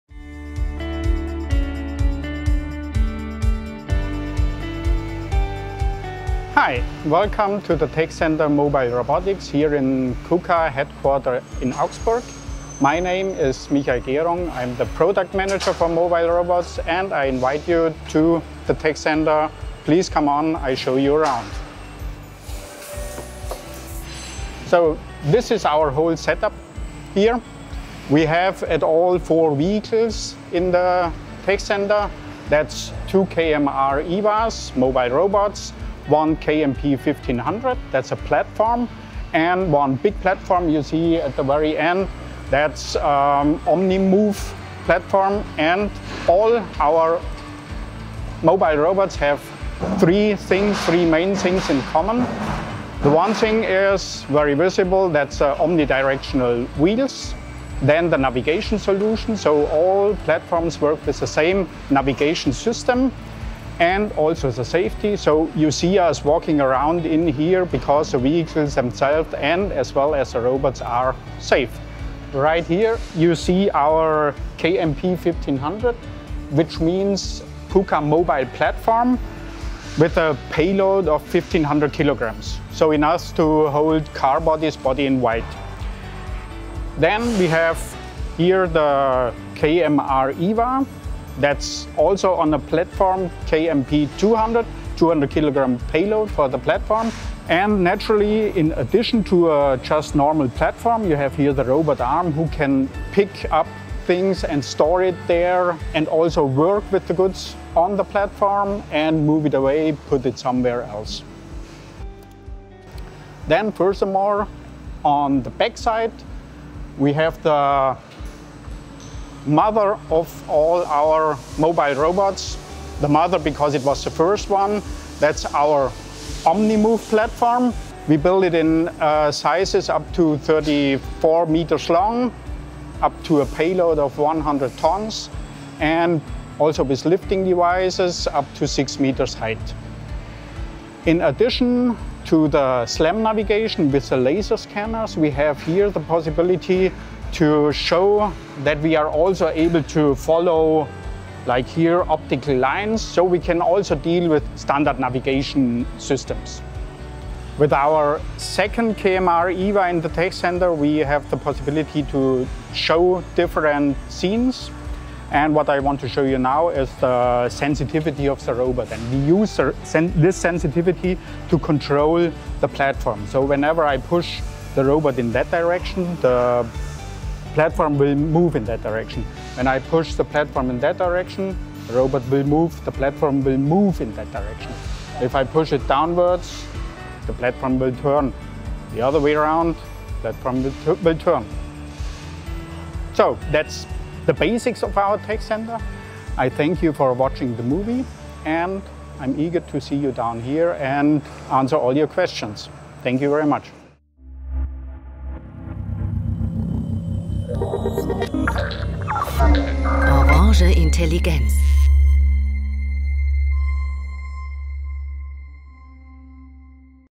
KUKA Mobile Robotics TechCenter Bavaria, Germany - Walkthrough
Recently KUKA has opened a series of TechCenters across the planet, designed to bring us closer to our customers and help them reduce their times to start of production with robotic automation. The facilities are set up to provide demonstrations in key industries, as well as provide a platform for customers to test and optimize their applications in real life before they disrupt their own factory floors with installation. The TechCenters are another step in KUKA's push to get closer to our partners and closely collaborate with them to solve their production challenges as well as help them implement new and innovative technology in the most efficient and quickest way possible.

In this video we walk you through the Mobile Robotics TechCenter at our HQ in Augsburg, Germany.